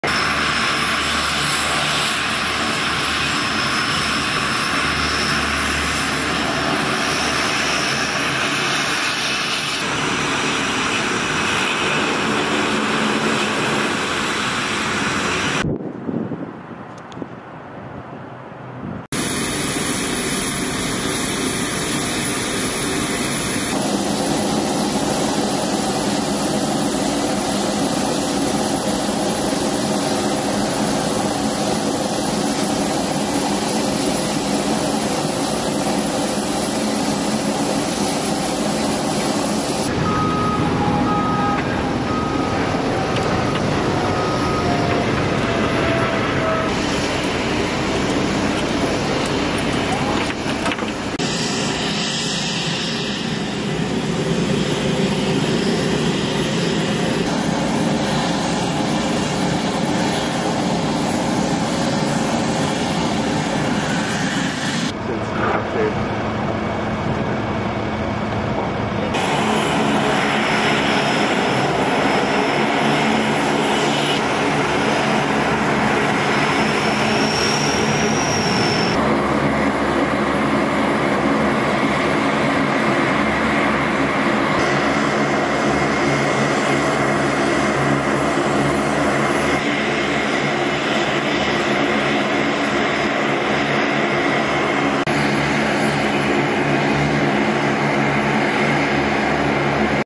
U.S. Marines Continue To Support Mindanao Relief Efforts Alongside Philippine Allies
U.S. Marines with Marine Aerial Refueler Transport Squadron (VMGR) 152, 1st Marine Aircraft Wing, alongside Philippine Air Force and Coast Guard service members prepare Department of Social Welfare and Development family food packs for transportation at Villamor Airbase, Pasay City, Philippines, Feb. 12, 2024.

PHILIPPINES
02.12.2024

At the request of the Government of the Philippines, the U.S. Marines of III Marine Expeditionary Force are supporting the U.S. Agency for International Development in providing foreign humanitarian assistance to the ongoing disaster relief mission in Mindanao. The forward presence and ready posture of III MEF assets in the region facilitated rapid and effective response to crisis, demonstrating the U.S.’s commitment to Allies and partners during times of need. (U.S. Marine Corps photo by Cpl. Alora Finigan)

Film Credits: Video by Cpl. Alora Finigan
1st Marine Aircraft Wing